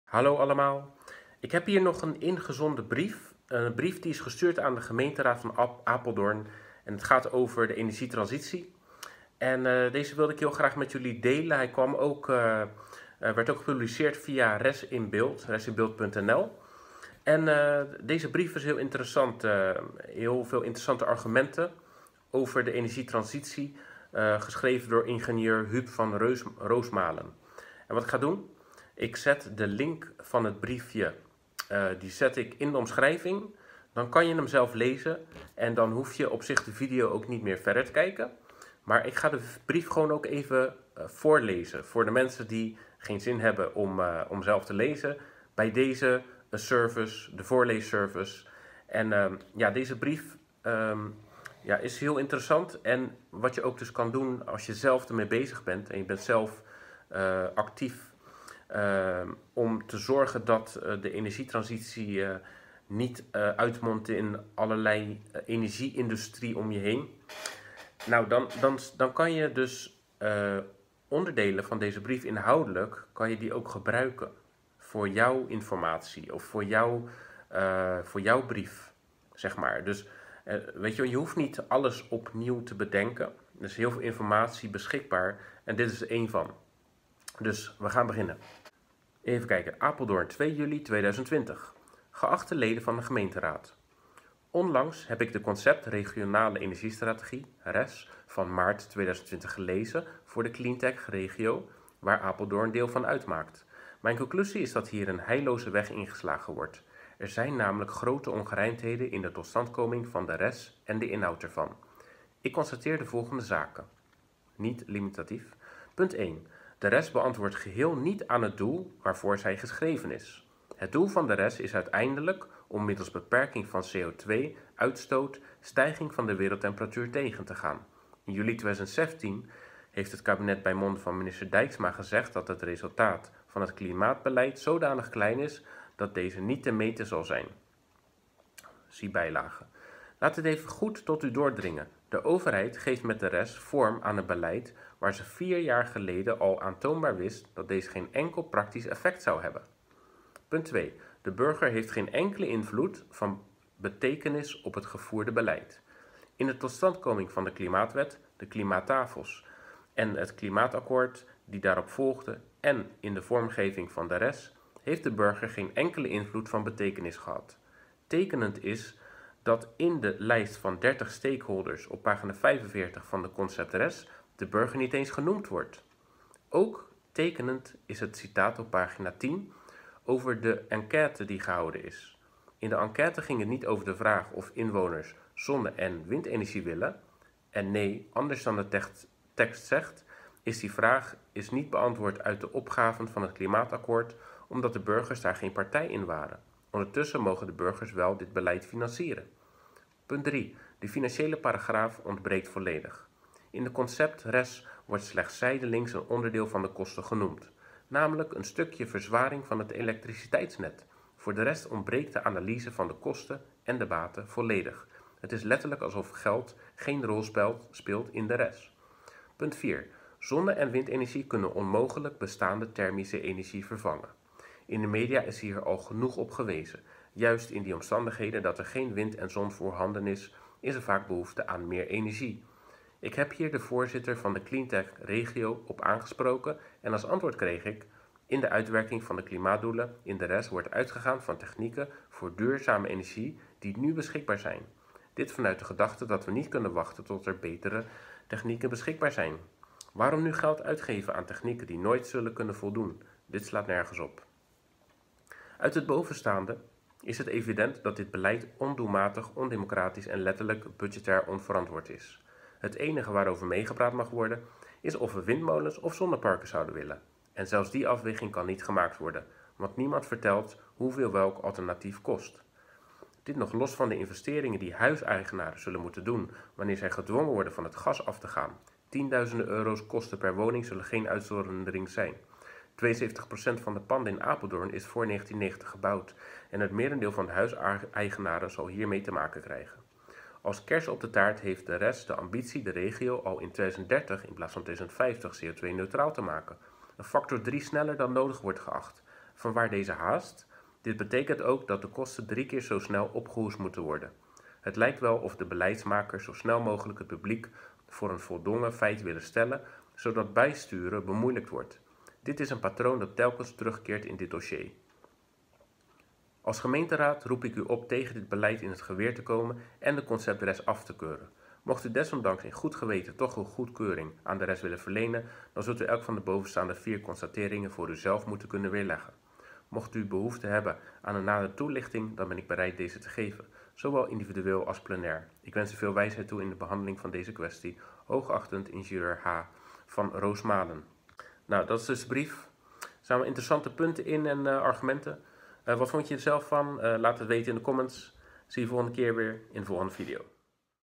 Zo maak je goed bezwaar tegen windmolens en zonneparken!
In deze video bekijken we een bezwaarbrief aan de gemeente Apeldoorn tegen het RES beleid dat bepaalt hoeveel windmolens en zonneparken er moeten komen in een regio.

Dit is een mooi voorbeeld van hoe je een bezwaar maakt met een aantal goede argumenten.

Maak er gebruik van als je ook bezwaar wilt maken!

Like, deel en aboneer!